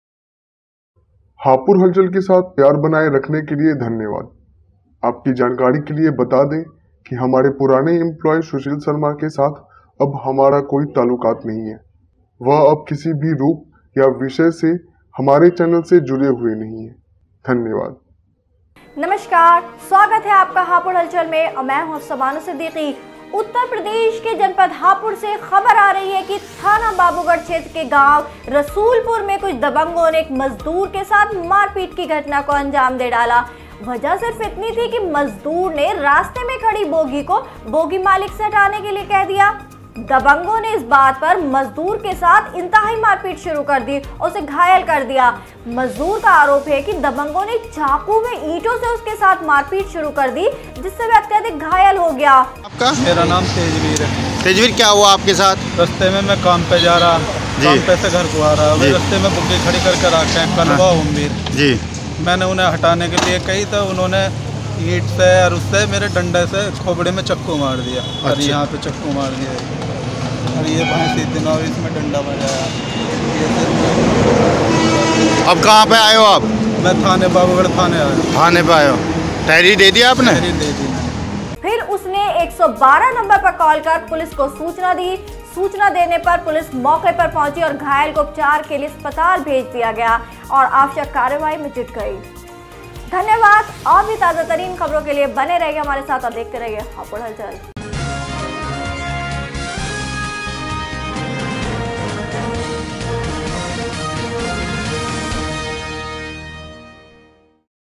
हापुड़ के बाबूगढ़ में मजदूर के साथ जमकर मारपीट।
उत्तर प्रदेश के जनपद हापुड़ के थाना बाबूगढ़ क्षेत्र के गांव रसूलपुर में दबंगों ने उस वक्त मारपीट शुरू कर दी। जब मजदूर  ने रास्ते मे खड़ी बुग्गी मालिक से बुग्गी हटाने को कहा।
 आपको बता दें कि थाना बाबूगढ़ क्षेत्र के गांव रसूलपुर में दबंगों ने एक मजदूर के साथ मारपीट कर  घायल कर दिया। वही मजदूर का आरोप है कि दबंगो ने चाकू व ईटों से उसके साथ मारपीट शुरू कर दी जिससे वह घायल हो गया पीड़ित ने डायल 112 पर कॉल कर पुलिस को सूचना दी बच्चा देते ही पुलिस मौके पर पहुंची घायल को उपचार के लिए भेज आवश्यक कार्रवाई में जुट गई।